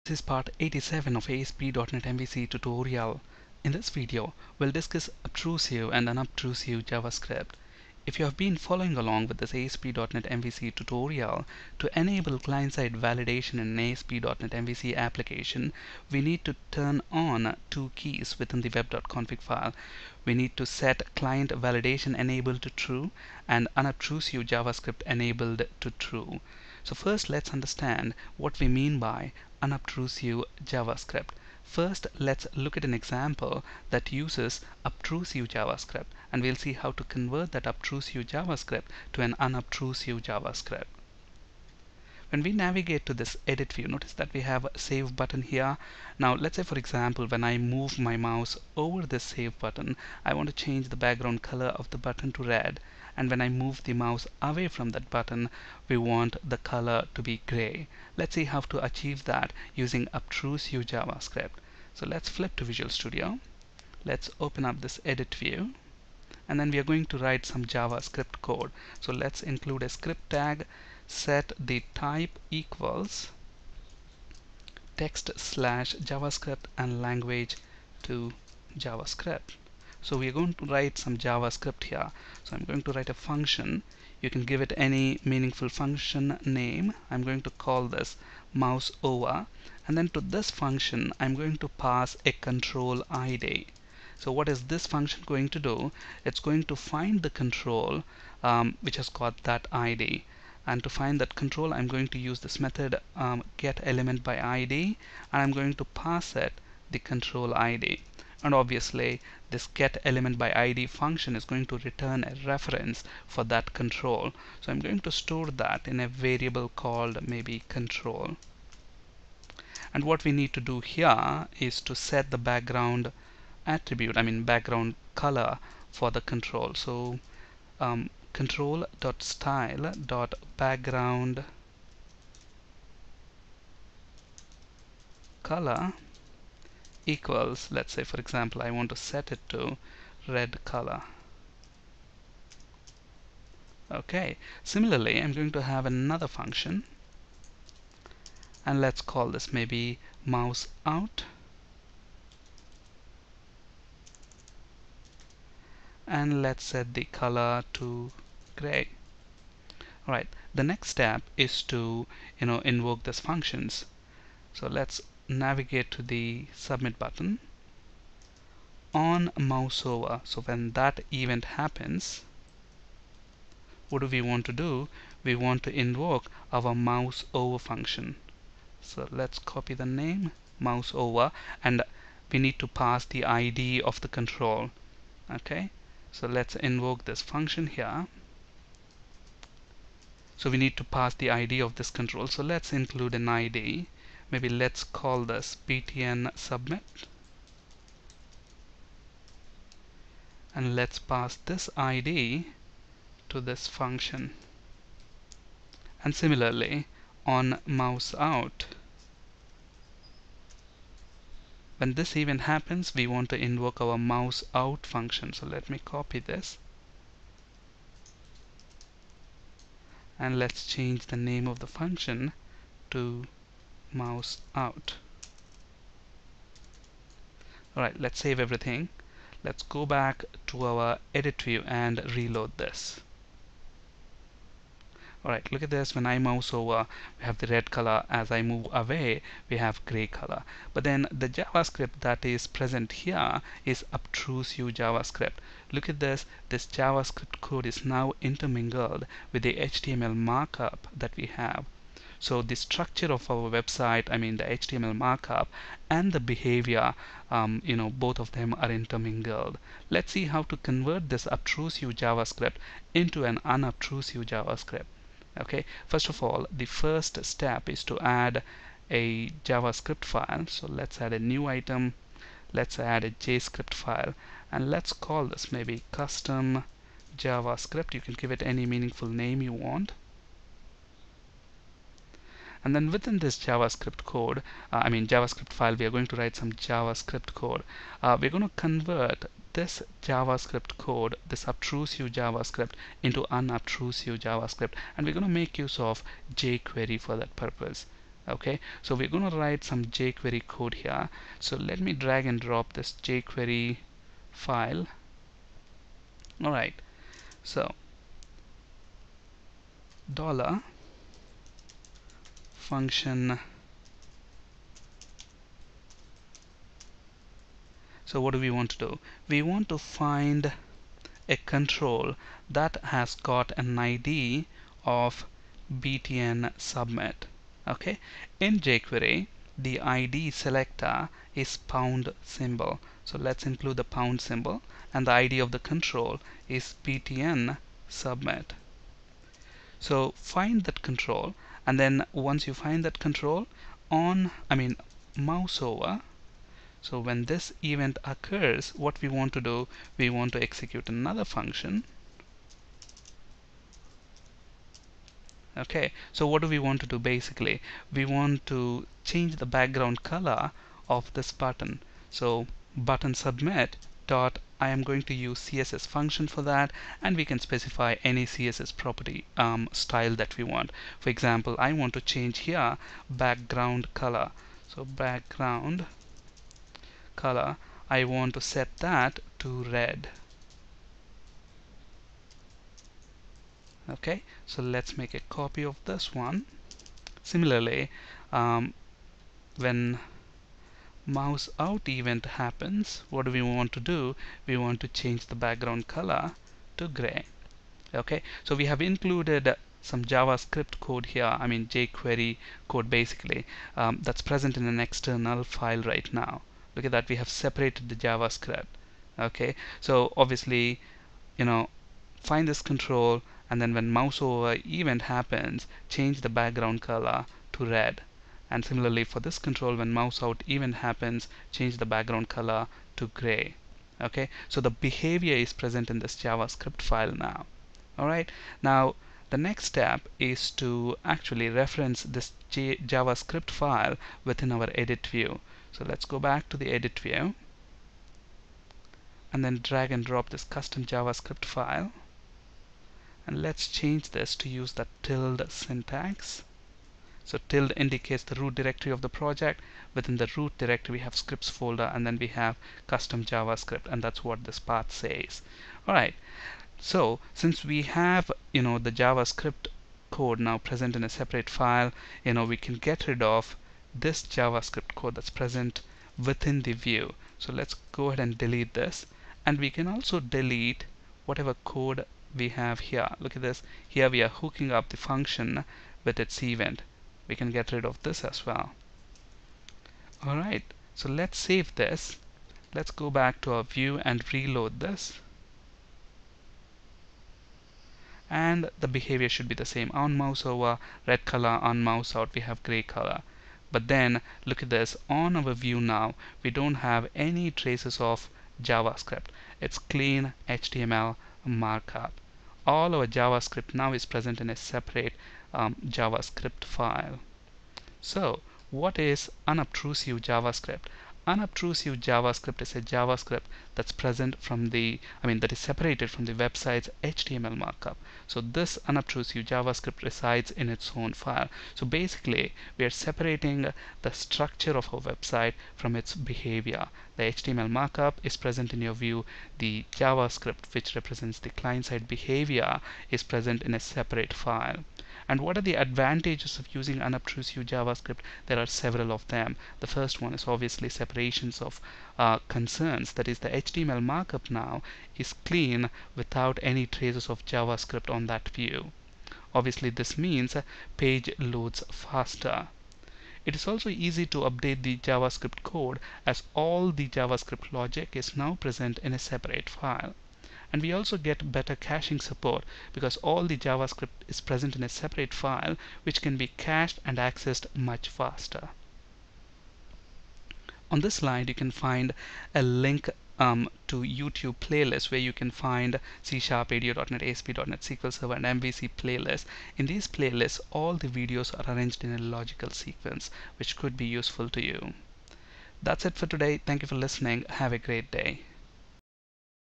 Part 87 What is Unobtrusive JavaScript
Link for code samples used in the demo

Link for csharp, asp.net, ado.net, dotnet basics, mvc and sql server video tutorial playlists

What is Unobtrusive JavaScript?
Unobtrusive JavaScript, is a JavaScript that is separated from the web site's html markup. There are several benefits of using Unobtrusive JavaScript. Separation of concerns i.e the HTML markup is now clean without any traces of javascript. Page load time is faster. It is also easy to update the code as all the Javascript logic is present in a separate file. We also get, better cache support, as all our JavaScript is now present in a separate file, it can be cached and accessed much faster.